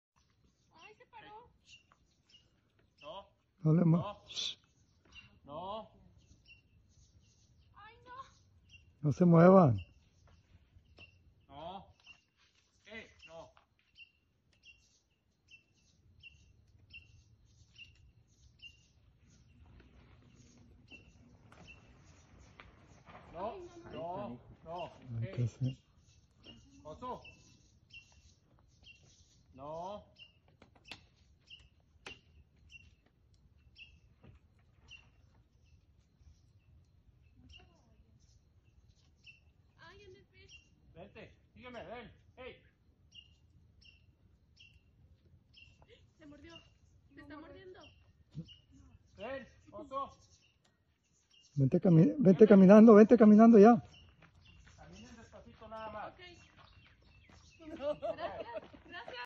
Woman takes selfie with black bear In Chipinque Ecological Park, Mexico
The hikers kept their cool when the curious black bear approached them from behind on a trail in Mexico.